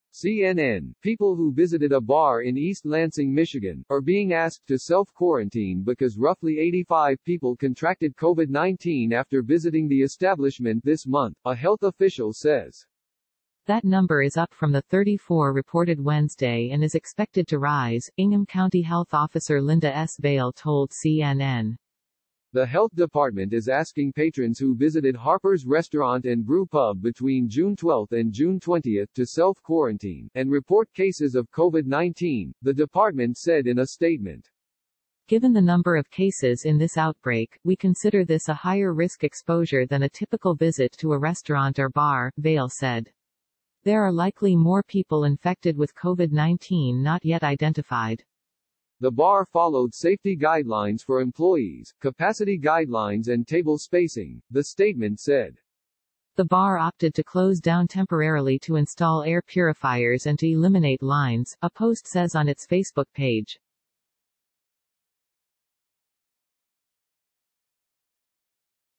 Patrons are asked to self-quarantine after about 85 people who visited a Michigan bar get Covid-19
(CNN)People who visited a bar in East Lansing, Michigan, are being asked to self-quarantine because roughly 85 people contracted Covid-19 after visiting the establishment this month, a health official says.